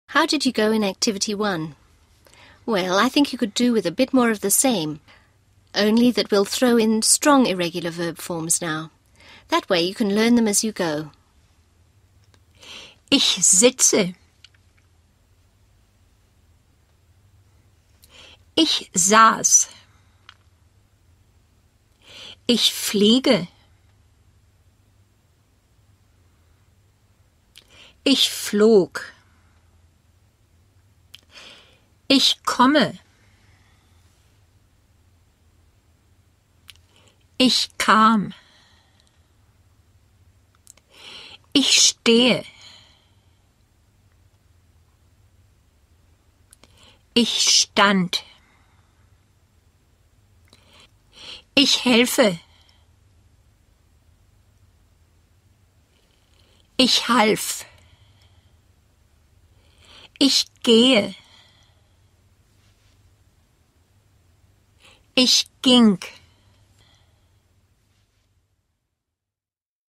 Simple Past — your turn 2
Simple Past digital booklet is printable material, including a script of the video, grammar tables, and some of the learning games

Ach, wie schön! Fotos aus der Schweiz
Fill in the blanks to reproduce the conversation between Karin and Peter.

Build your vocabulary by working with words from the conversation between Jutta and Marianne!

Put the phrases from the conversation in the right order.